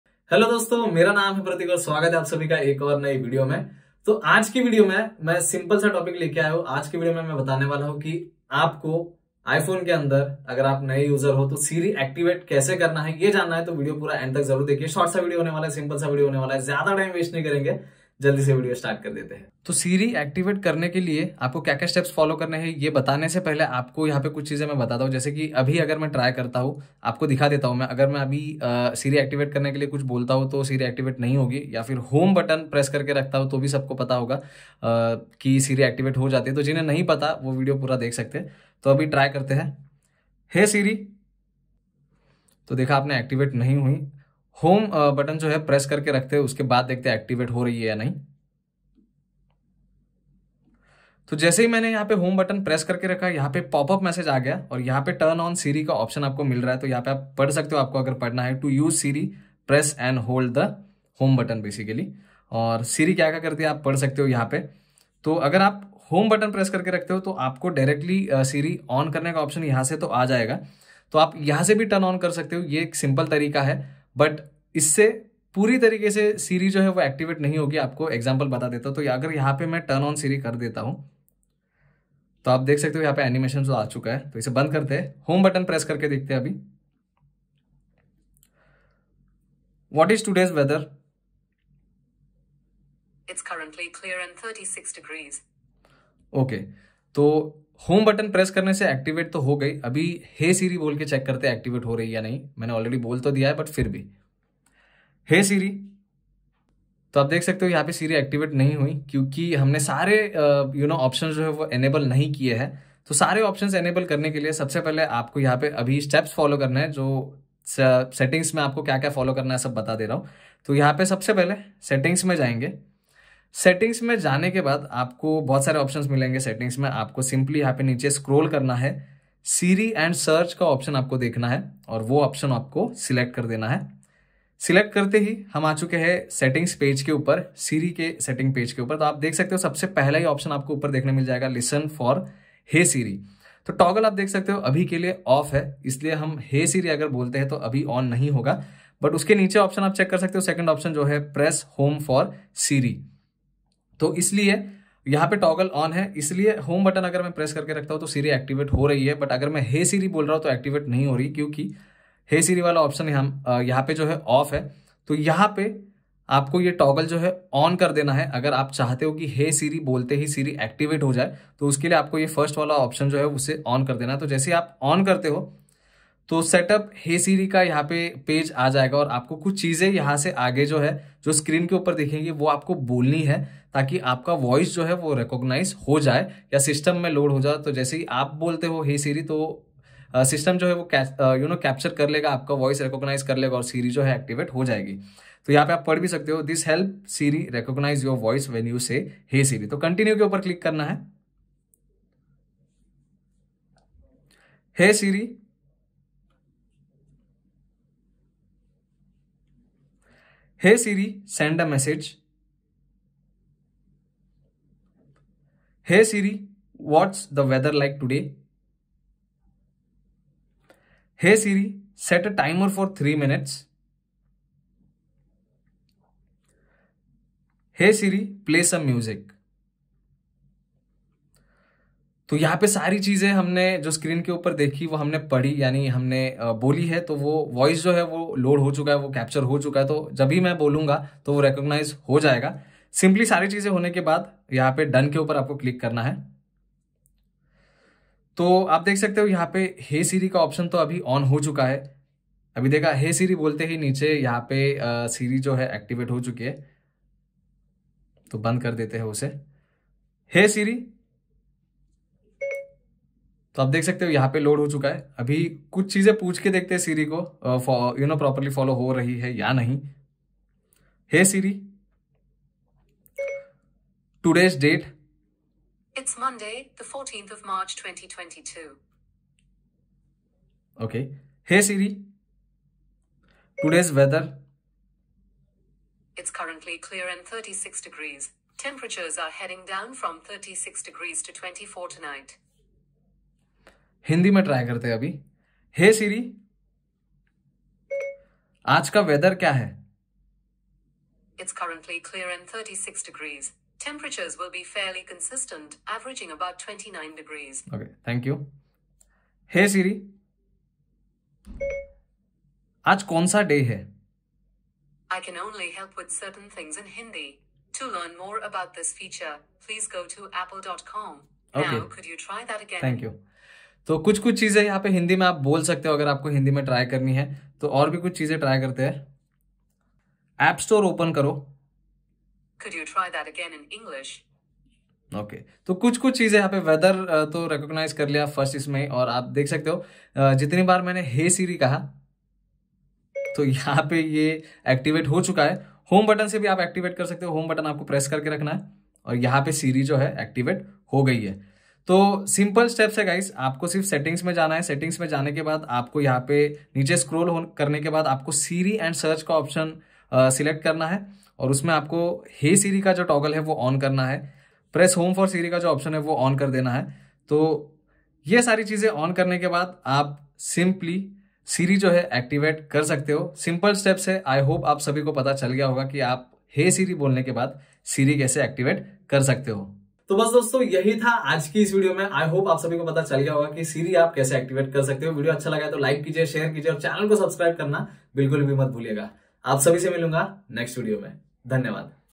iPhone में SIRI कैसे चलाएं? | How to Setup SIRI on iPhone?
How to activate and setup SIRI on an iPhone? SIRI is a digital assistant that you can trigger on an iPhone with a few gestures. But, before you start using SIRI and ask her questions, you need to set it up. Let me show you how to set up SIRI on any iPhone in this simple Hindi video.